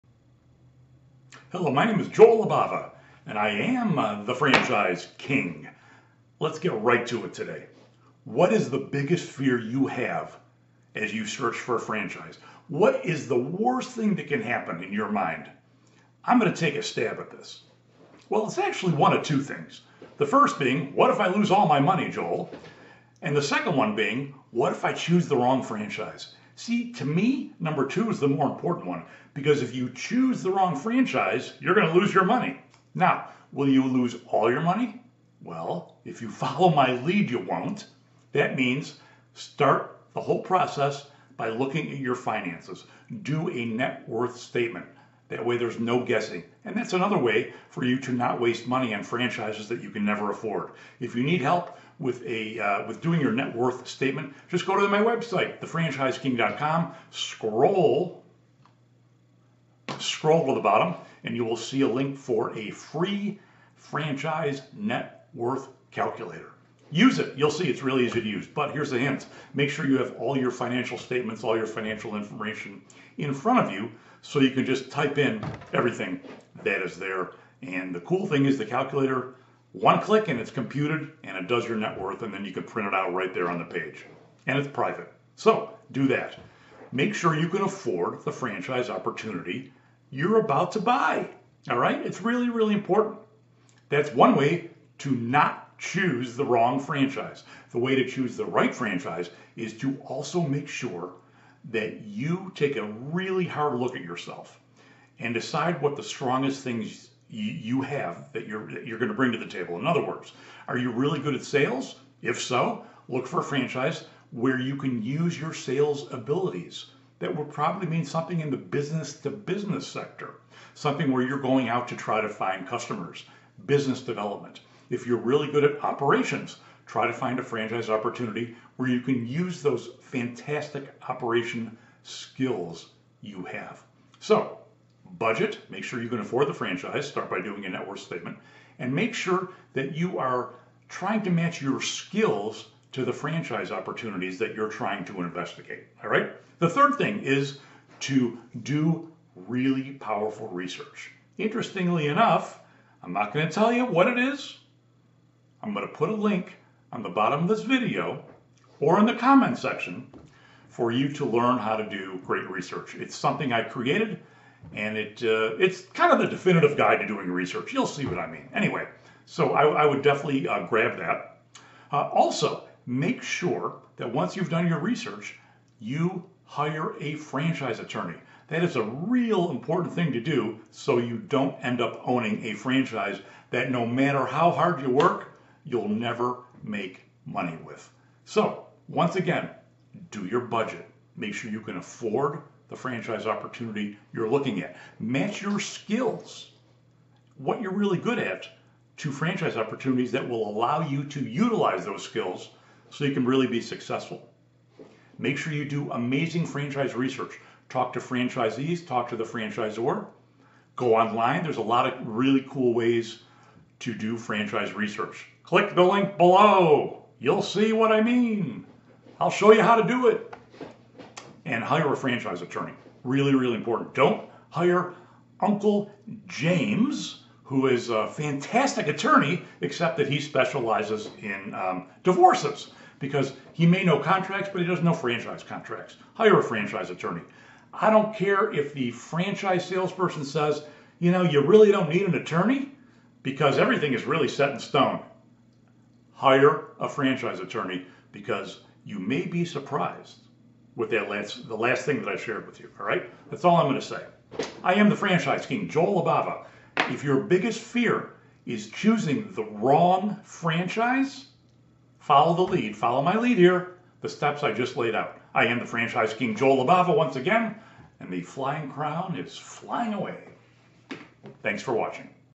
How to Take The Fear Out of Choosing The Wrong Franchise
In this video, The Franchise King® shares 4 things you can do to remove the fear of making a poor choice in a franchise to own.
Tip: make sure you check out his Franchise Research Guide!

Joel Libava's franchise videos provide actionable tips and advice for today's future franchise business owners.